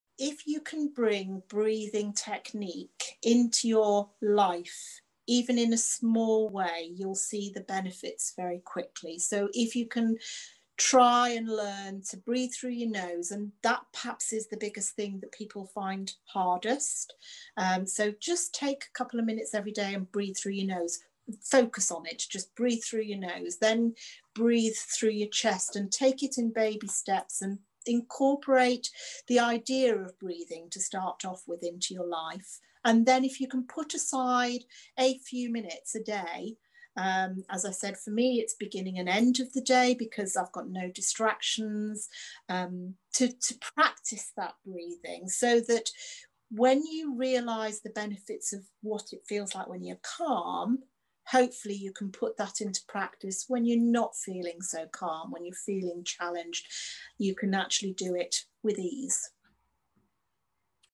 INSTANT INSIGHT | Stop and Breathe
I often find the closing thoughts of my podcast guests are so brilliantly thoughtful and incisive that I want to return to them again and again.  So I decided I would share them as little soundbites that we can all use to inspire us. I hope this instant’s insights series inspires you.

Today’s inspiration comes from Mel Holliday.